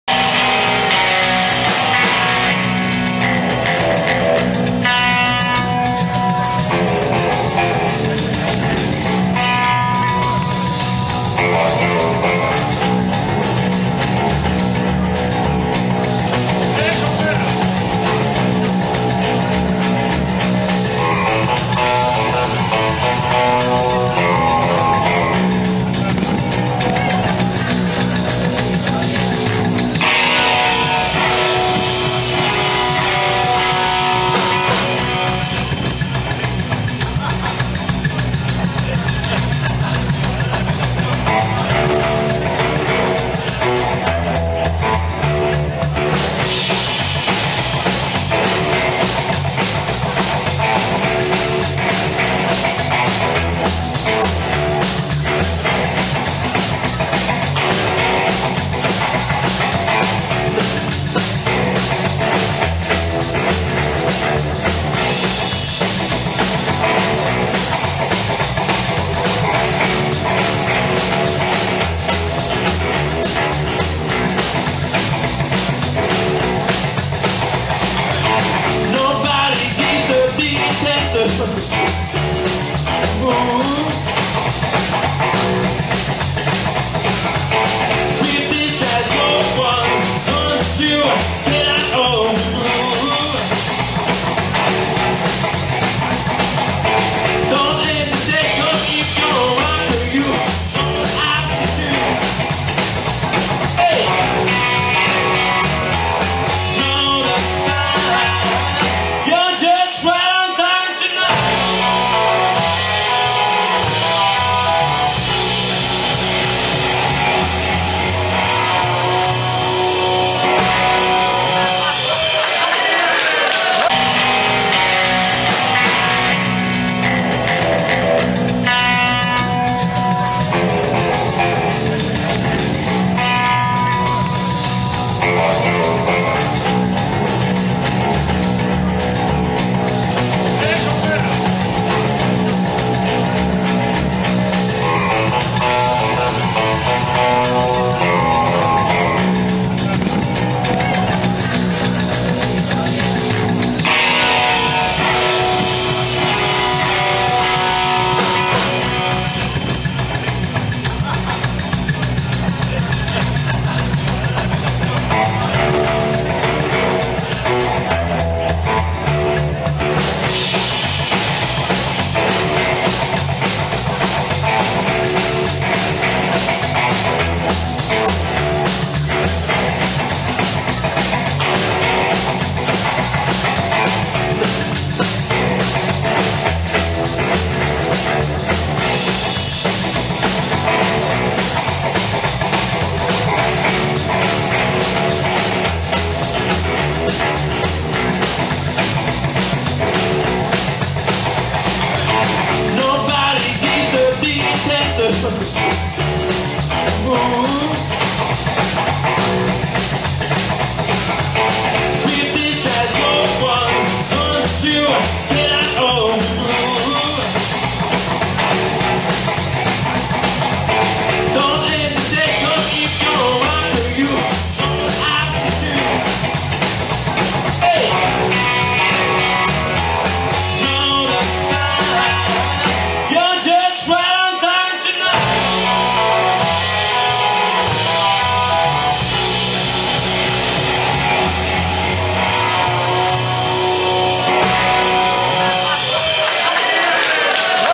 Impro Rock Dance
Api Ness danse en improvisation accompagné par un groupe de rock surdoué dans la garage du célèbre artiste Franck Defieu rénovateur de génie des porchs de luxe